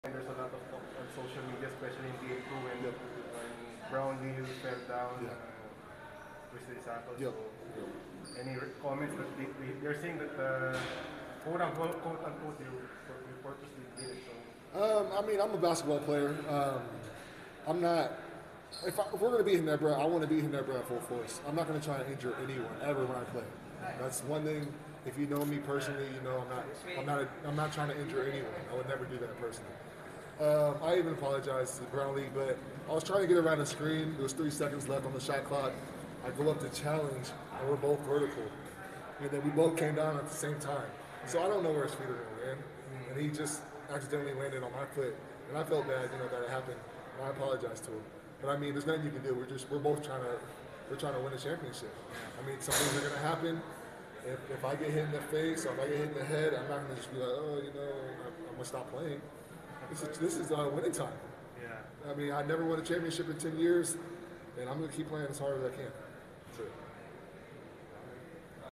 Cliff Hodge on fans' allegations of playing dirty, trying to hurt Justin Brownlee on purpose
Meralco forward Cliff Hodge speaks about online talk against him